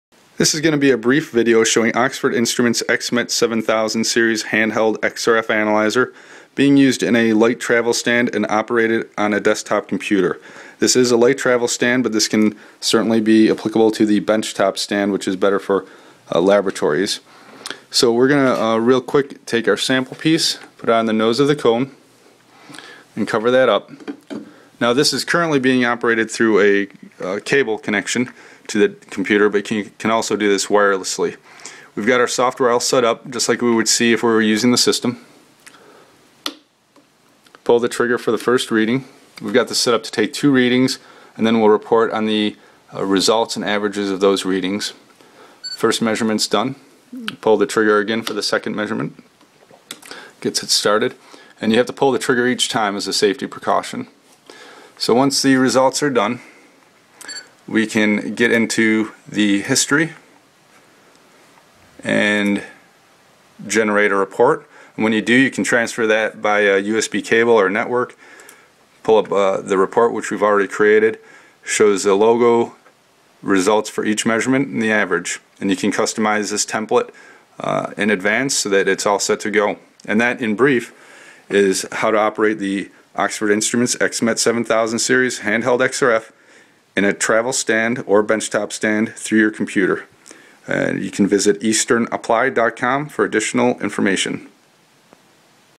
Handheld XRF Operated by Computer
Eastern Applied associates demonstrate the X-Met7000 series Handheld XRF from Oxford Instruments. This video shows the analyzer in a travel stand and operated by computer. Contact Eastern Applied Research to learn more about this function.